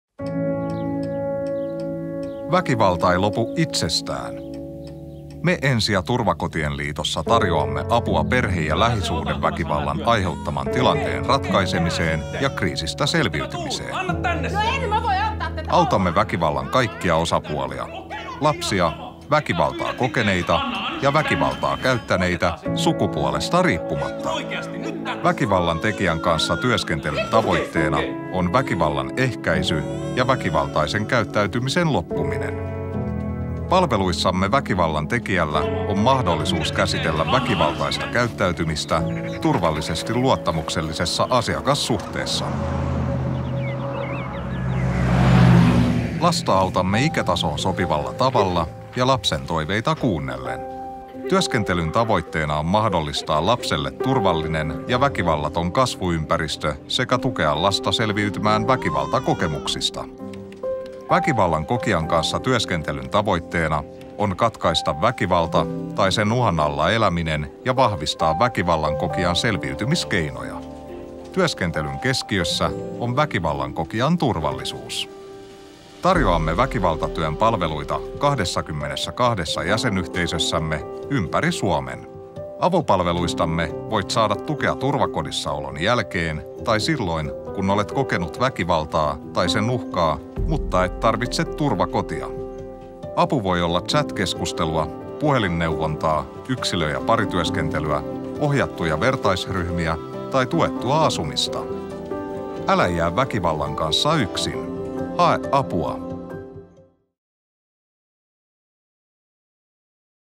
Väkivaltatyön avopalvelut Ensi- ja turvakotien liitossa
Väkivallasta on mahdollista päästä eroon ja selviytyä.

Ensi- ja turvakotien liiton väkivaltatyön avopalveluista saat apua perhe- ja lähisuhdeväkivallan aiheuttaman tilanteen ratkaisemiseen ja kriisistä selviytymiseen. Apua on tarjolla kaikille väkivallan osapuolille: lapsille, väkivaltaa kokeneille ja väkivaltaa käyttäneille, sukupuolesta riippumatta.

Tarjoamme väkivaltatyön palveluita 22 jäsenyhteisössämme ympäri Suomen. Avopalveluistamme voit saada tukea turvakodissa olon jälkeen tai silloin kun olet kokenut väkivaltaa tai sen uhkaa, mutta et tarvitse turvakotia. Apu voi olla puhelinneuvontaa, keskusteluapua, ohjattu vertaisryhmä tai tuettua asumista. Voit tulla myös chatteihimme keskustelemaan nimettömästi.

Älä jää väkivallan kanssa yksin, hae apua.